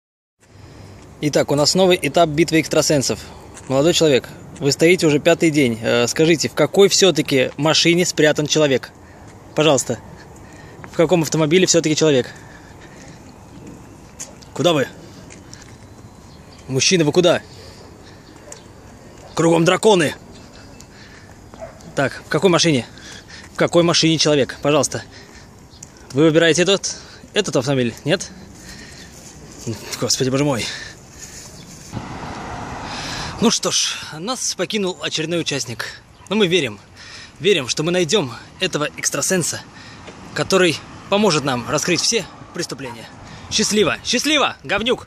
Битва экстрасенса
решил проверить экстрасенсорные способности обычного парня